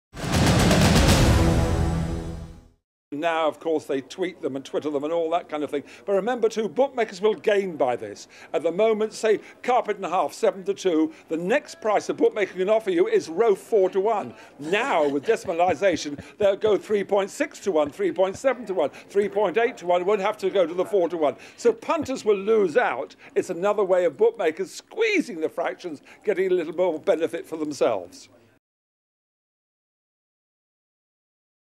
Horse racing to get a make over
John McCririck explains why the changes to horse racing had to come.